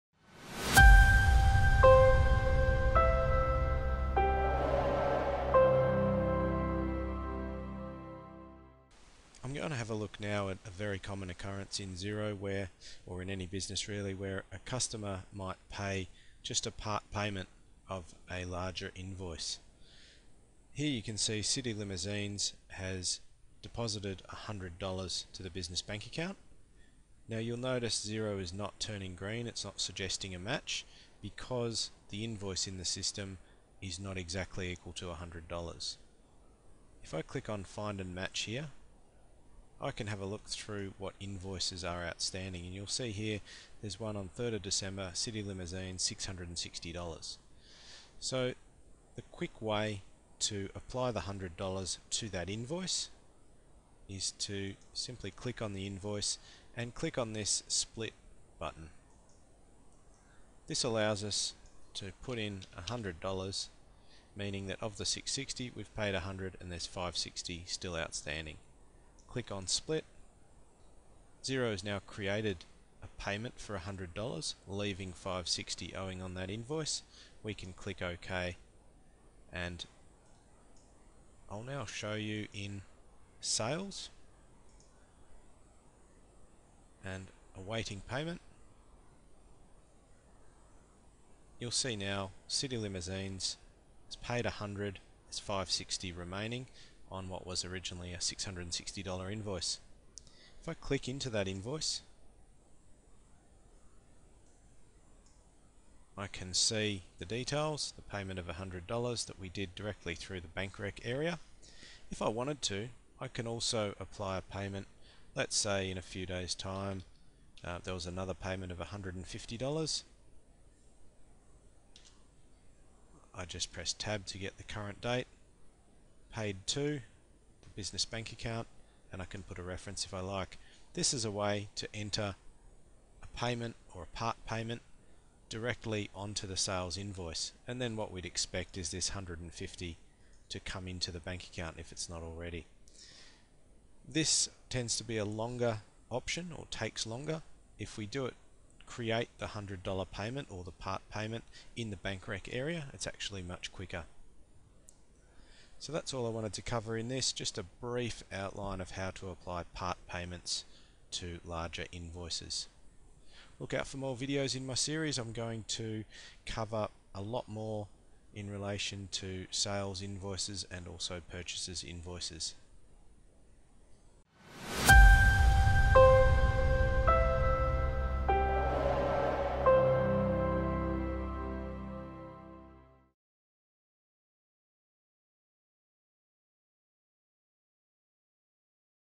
Apply Part Payment to a Sales Invoice
In this video, Geoff Trabant from Easy Online Bookkeeping, shows you how to apply part payment to a sales invoice in Xero.